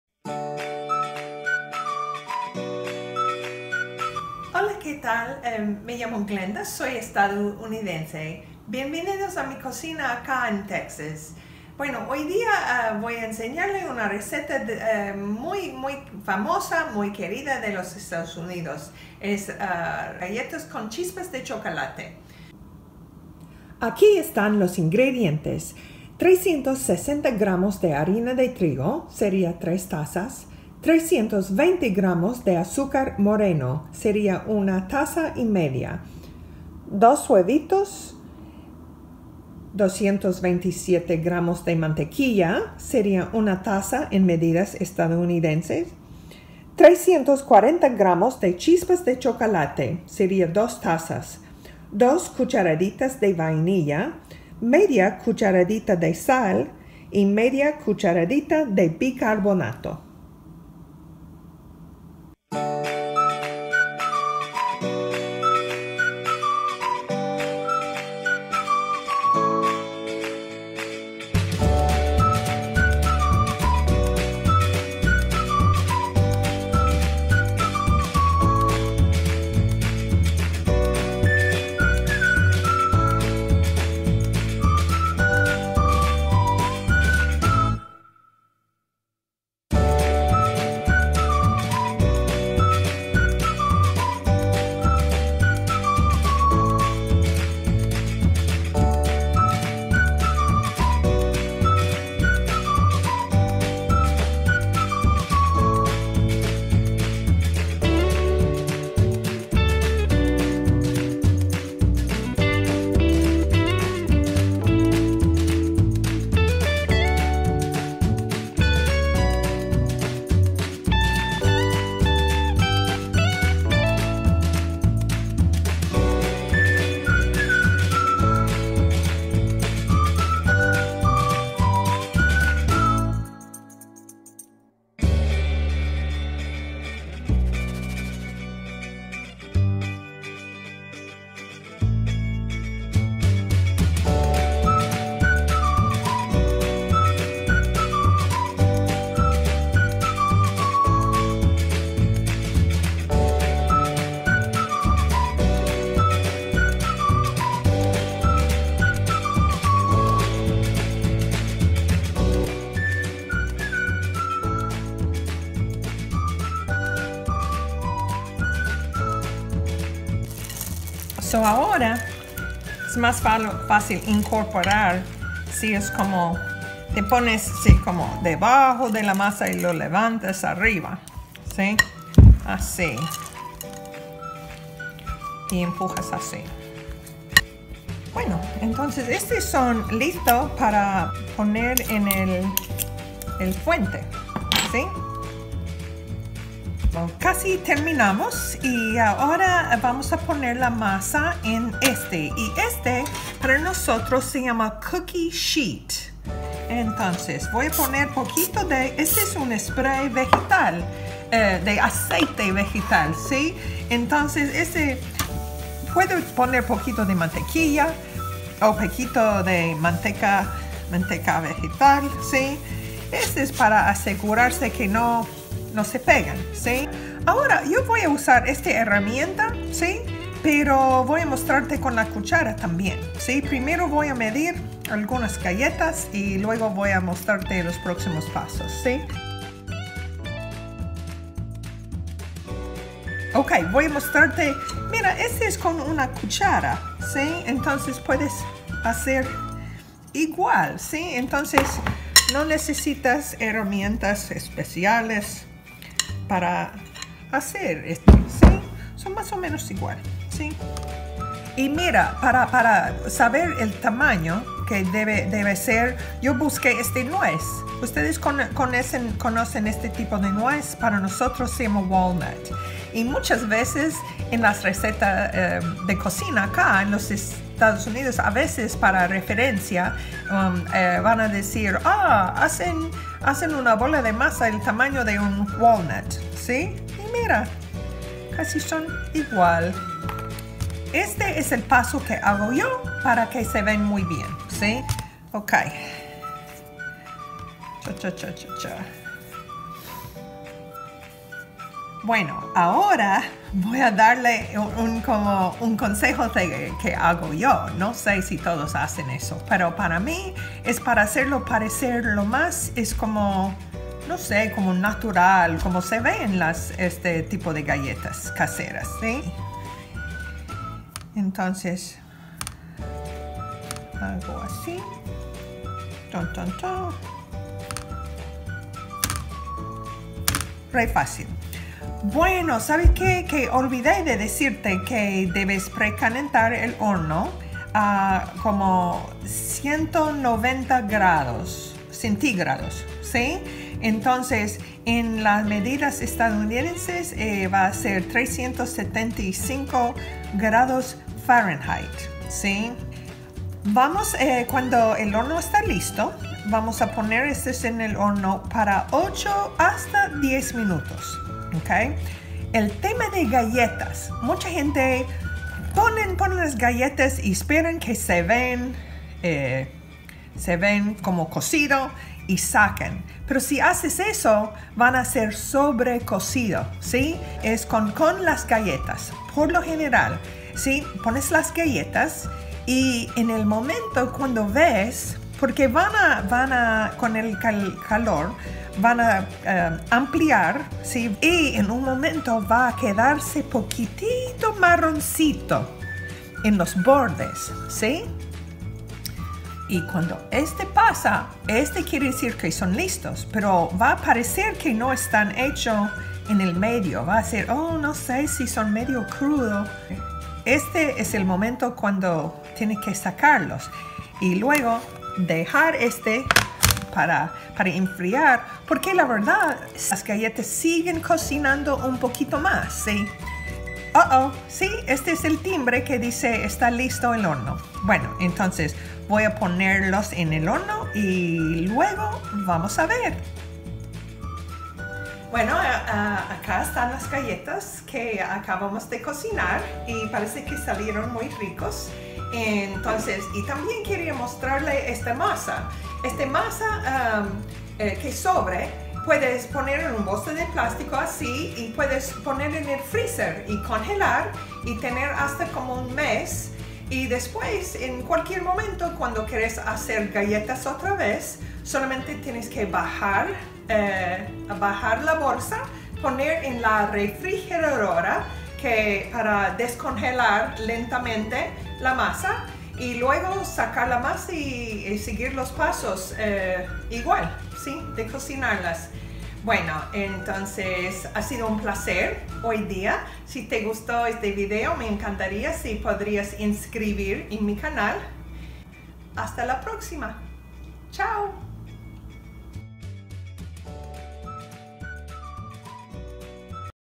Galletas con Chispas de Chocolate ¡Las Hacemos Así en Mi Casa!
¿Quieres hacer la galleta mas popular en los EE.UU? Me da mucho gusto compartir una receta para galletas con chispas de chocolate caseras que preparo a menudo. El video tiene instrucciones paso a paso como prepararlos. A mi familia y amigos les gustan mucho. Ingredientes: 3 tazas (360 gr.) de harina, 1/2 cucharadita sal, 1/2 cucharadita bicarbonato, 1 1/2 taza (320 gr.) de azúcar moreno, 1 taza (227 gr.) de mantequilla, 2 huevos, 2 cucharaditas de eséncia de vainilla, 2 tazas (340 gr.) de chispas de chocolate, opcional: 2 tazas (250 gr.) de nueces picadas (pecans)

In English: 3 cups flour, 1/2 tsp. salt, 1/2 tsp. soda, 1 1/2 cups brown sugar, 1 cup butter, 2 eggs, 2 tsp. vanilla extract, 2 cups (12 oz.) chocolate chips, optional: 2 cups chopped pecans

375°F / 170°C